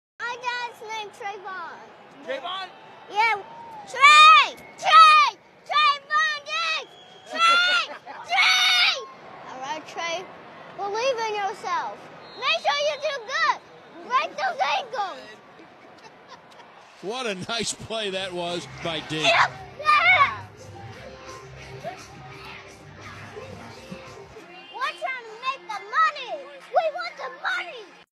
Aaiden Diggs is his dad’s biggest fan ❤️
Tevon Diggs' son mic'd up

(Via NFL Films)